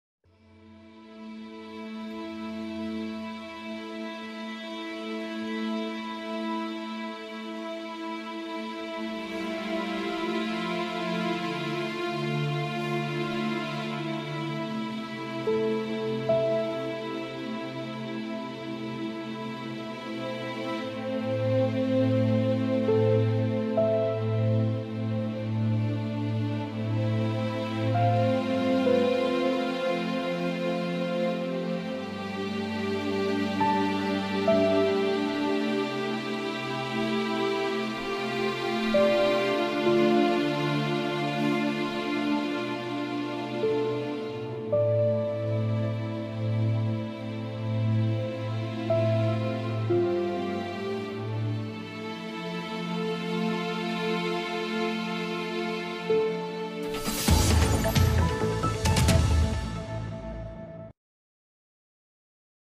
Esed Rejimi, Rusya'nın hava desteğiyle İdlib'de ele geçirdiği Maarratünnuman'ı hayalet kente çevirdi
￼Görüntülerde, ilçe merkezindeki saldırıların yoğunluğu gözler önüne seriliyor.